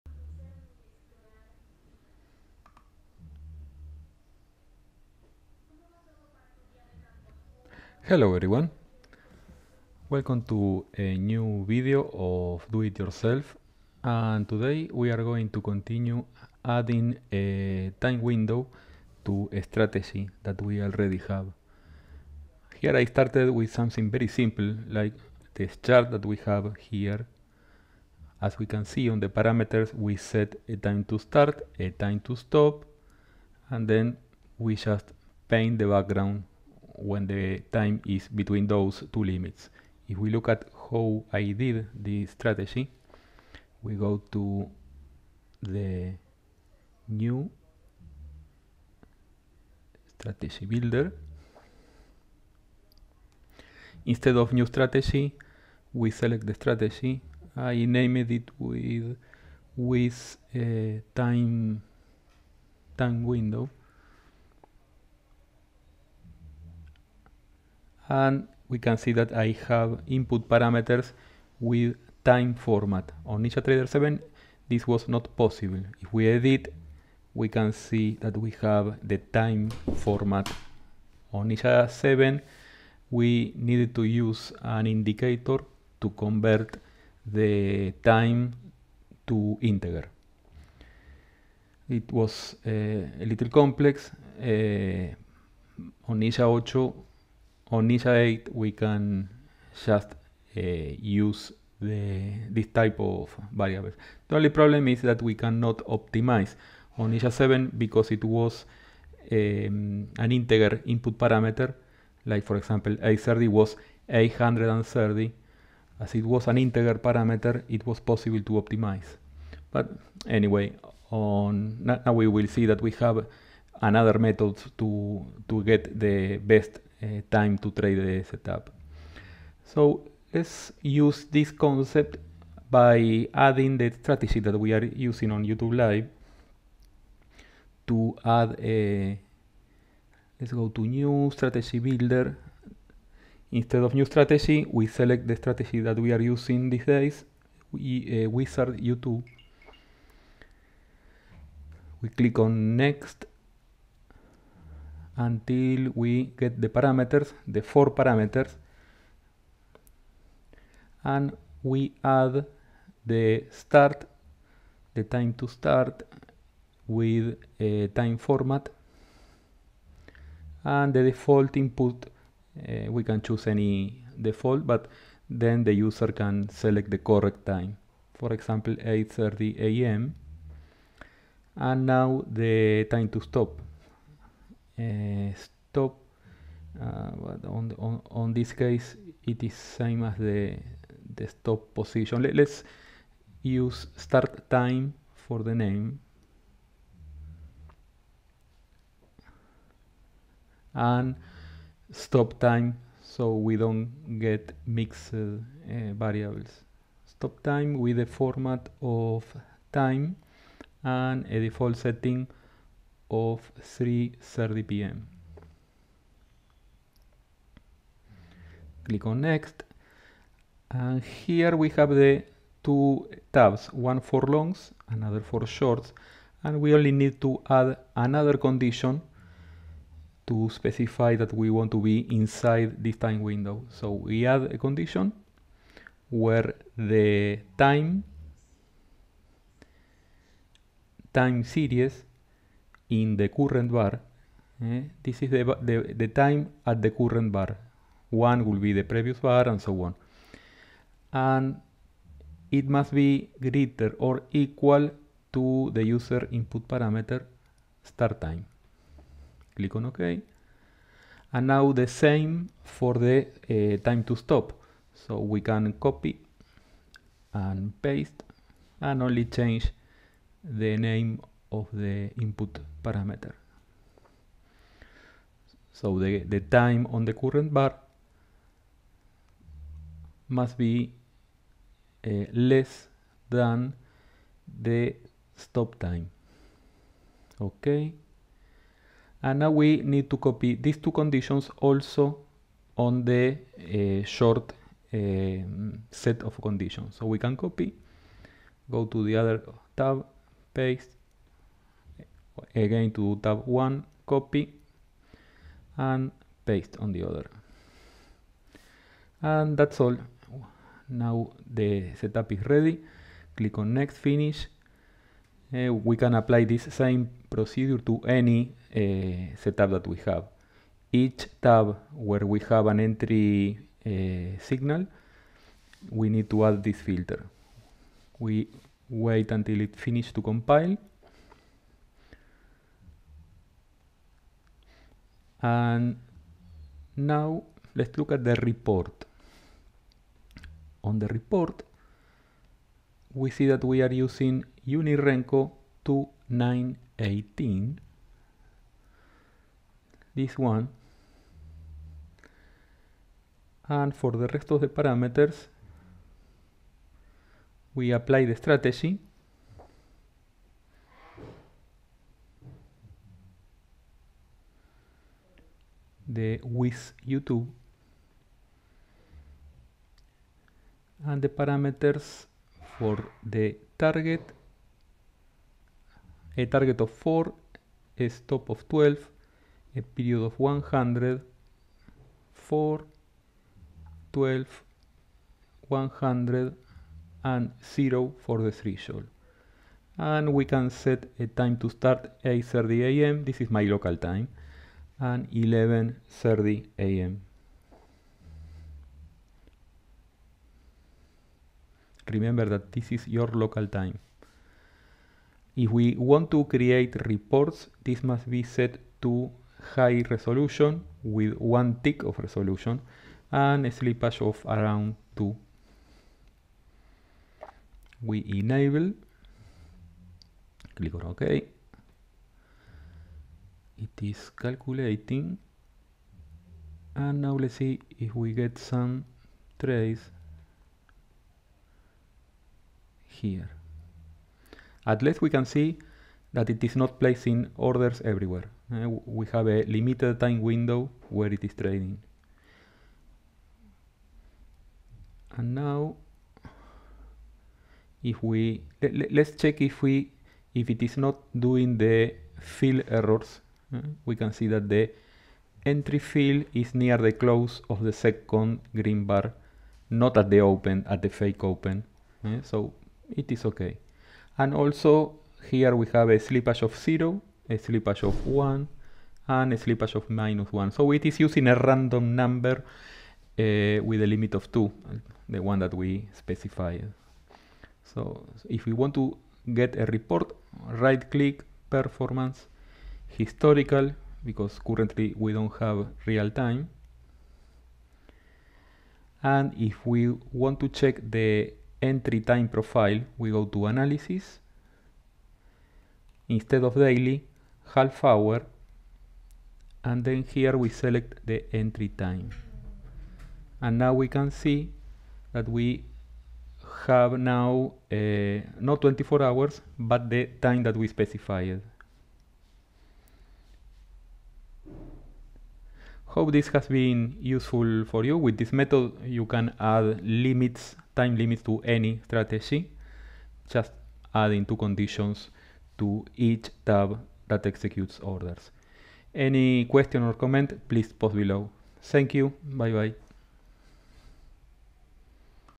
Do It Yourself - Add Time Trading Limits with NinjaTrader Builder
On this video I show how to add Time Windows Limits to a Strategy designed with Strategy Builder on Ninjatrader 8.